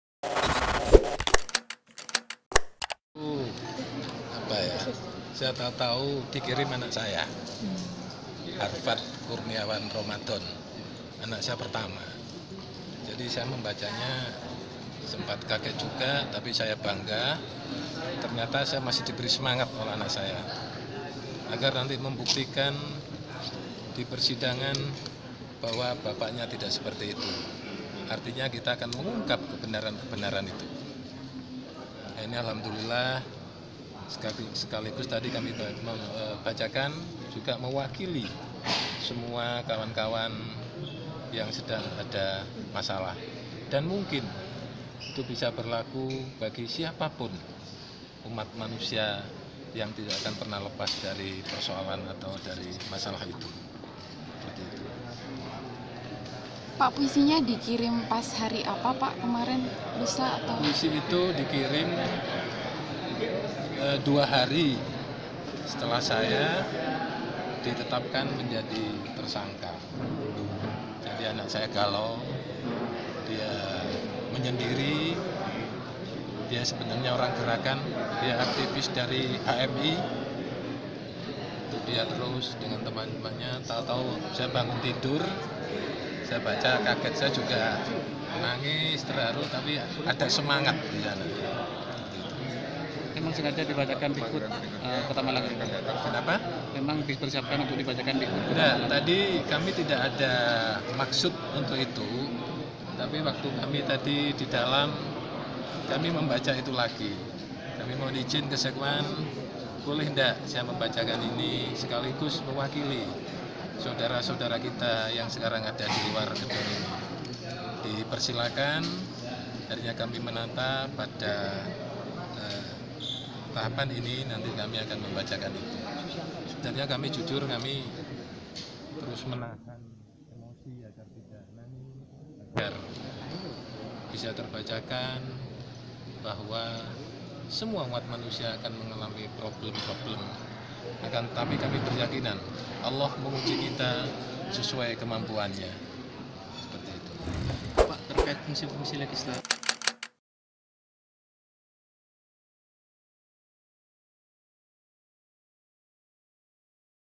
Abdul Hakim Tutup Sidang Paripurna Dengan Air Mata
MALANGTODAY.NET - Suasana sidang paripurna istimewa DPRD Kota Malang dengan agenda peringatan HUT Kota Malang ke 108 terasa berbeda dengan sebelumnya. Bahkan di akhir sidang, ketua DPRD Kota Malang, Abdul Hakim menutuonya dengan sebuah pusi dari sang anak yang dibacakan dengan iringan derai air mata.